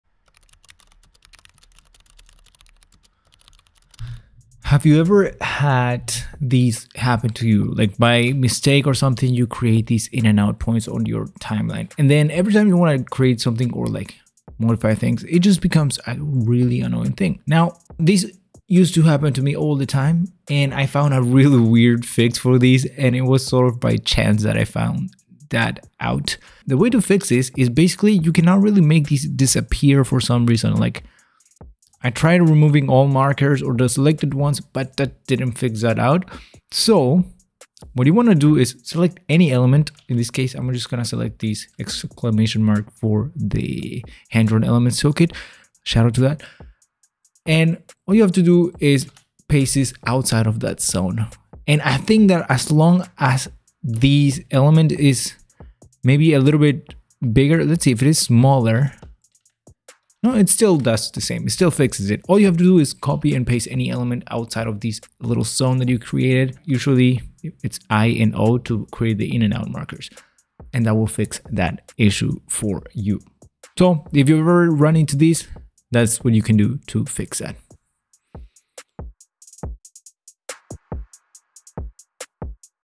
How to REMOVE IN and OUT Points in DaVinci Resolve
Remove In and Out points in DaVinci Resolve.

Or on the SUALVI store.

00:00 DaVinci Resolve In and Out Points
00:03 Clear In and Out points in DaVinci Resolve